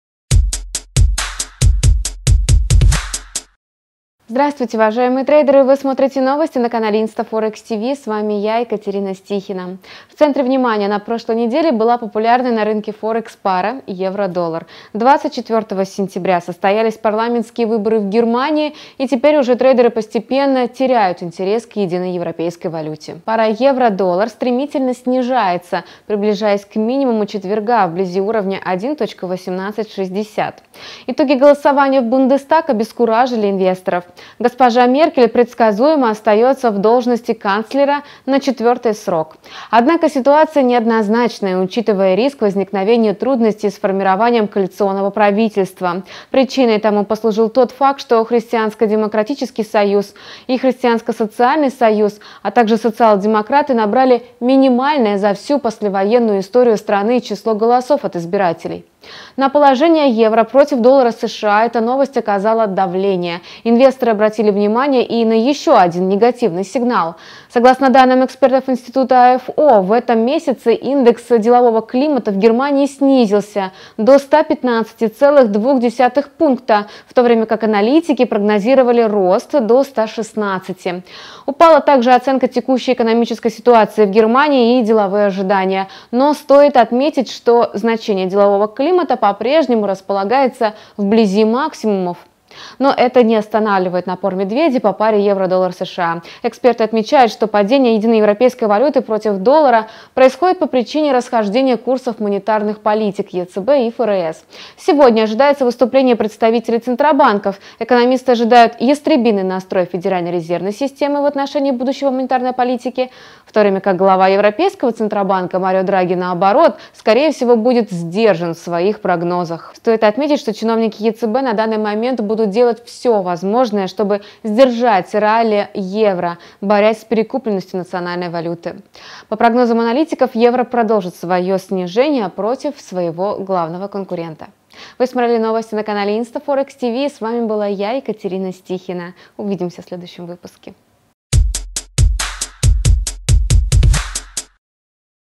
На очереди медвежий тренд по EUR/USD (25.09.2017)
В центре внимания на прошлой неделе была популярная на рынке Форекс пара евро/доллар. 24 сентября состоялись парламентские выборы в Германии, и теперь уже трейдеры постепенно теряют интерес к единой европейской валюте.
Пара EUR/USD стремительно снижается, приближаясь к минимуму четверга вблизи уровня 1.1860. Итоги голосования в Бундестаг обескуражили инвесторов.
Госпожа Меркель предсказуемо остается в должности канцлера на 4-й срок подряд. Однако ситуация неоднозначная, учитывая риск возникновения трудностей с формированием коалиционного правительства. Причиной тому тот факт, что Христианско-демократический союз, Христианско-социальный союз, а также социал-демократы набрали минимальное за всю послевоенную историю страны число голосов от избирателей. На положение евро против доллара США эта новость оказала давление. Инвесторы обратили внимание еще на один негативный сигнал.
Согласно данным экспертов института IFO, в этом месяце индекс делового климата в Германии снизился до 115,2 пункта, в то время как аналитики прогнозировали рост до 116. Упала также оценка текущей экономической ситуации в Германии и деловые ожидания. Но стоит отметить, что значение делового климата по-прежнему располагается вблизи максимумов. Но это не останавливает напор медведей по паре евро/доллар США.
Эксперты отмечают, что падение единой европейской валюты против доллара США происходит по причине расхождения курсов монетарной политики ЕЦБ и ФРС. Сегодня ожидается выступление представителей центробанков. Экономисты верят в ястребиный настрой Федеральной резервной системы в отношении будущего монетарной политики, в то время как глава Европейского Центробанка Марио Драги, наоборот, скорее всего, будет сдержан в своих прогнозах.
Стоит отметить, что чиновники ЕЦБ на данный момент будут делать все возможное, чтобы сдержать ралли евро, борясь с перекупленностью национальной валюты. По прогнозам аналитиков, евро продолжит снижение против своего главного конкурента.